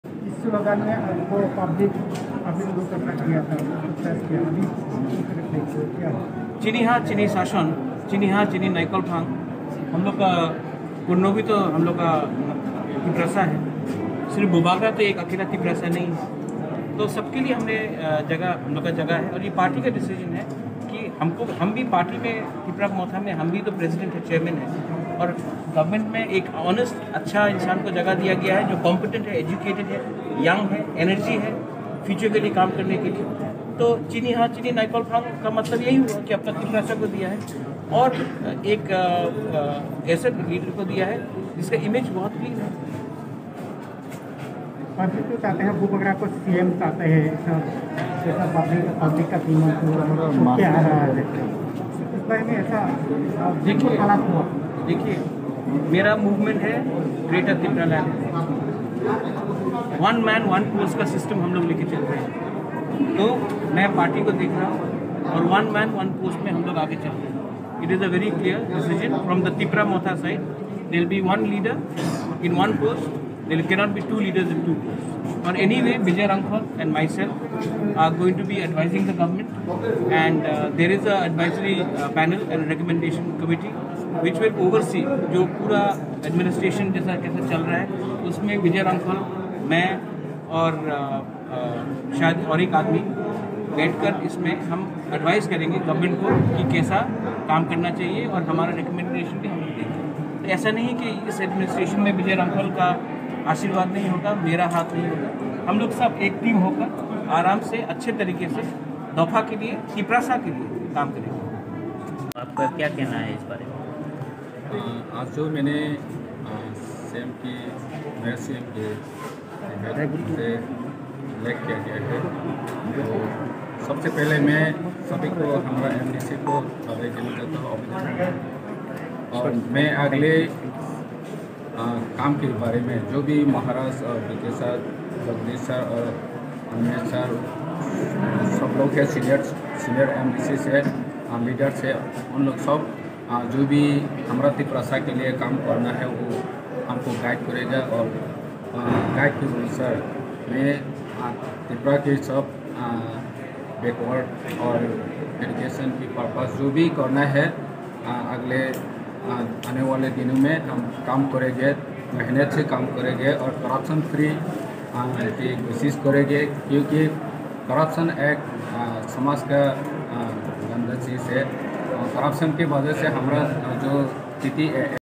BUBAGRA NI THANSA NAHAR MUNG GREATER TIPRALAND SAKHA BUBAGRA
Bubagra ni THANSA sa ni nahar mung Greater Tipraland hing noi sa BUBAGRA
CM ong ya Fano Adviser tng gwi se
Samong khwlai nai hin noi
Sa kha BUBAGRA